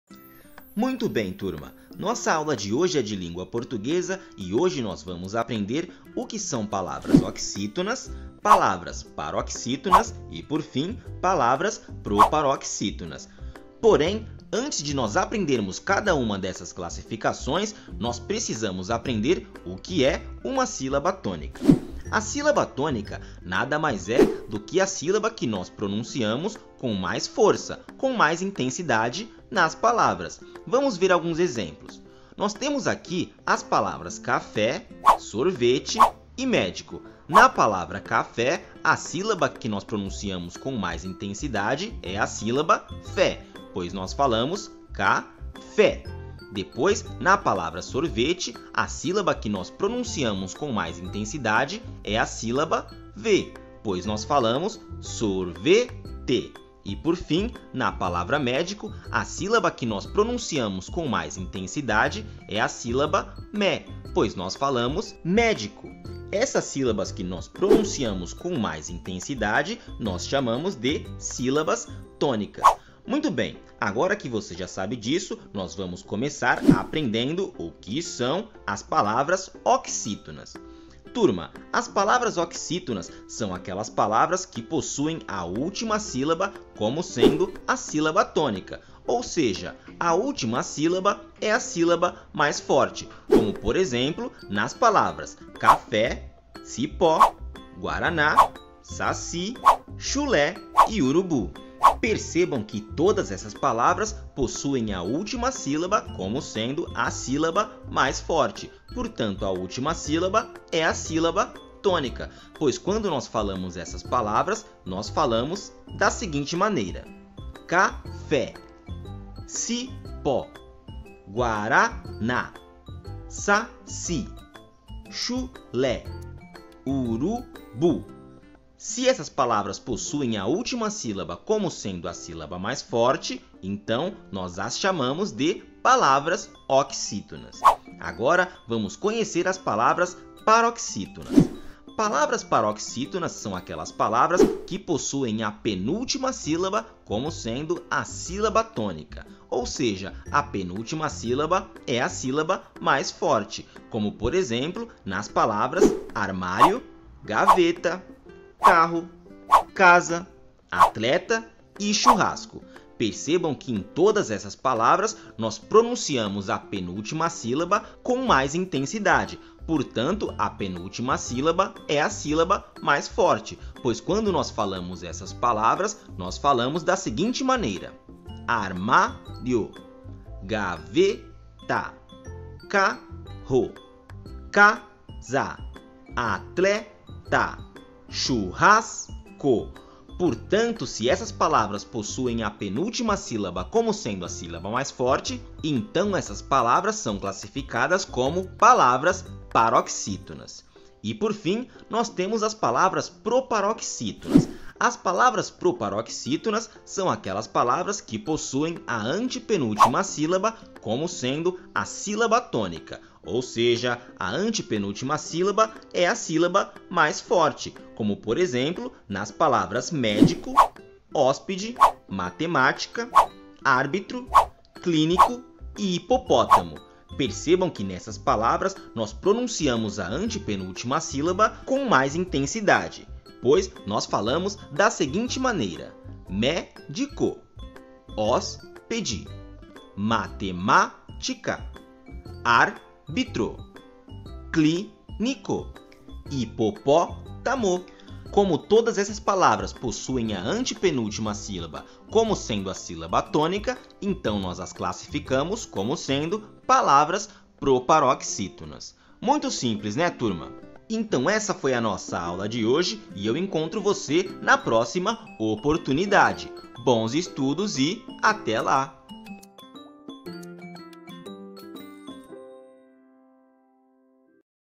SÍLABAS TÔNICAS | OXÍTONAS, PAROXÍTONAS E PROPAROXÍTONAS
Nessa aula vamos aprender sobre a tonicidade das palavras e aprender a classificar as palavras em OXÍTONAS, PAROXÍTONAS e PROPAROXÍTONAS de acordo com o local dessas sílabas.

Bons estudos e até a próxima aula. 📚📝📒✏️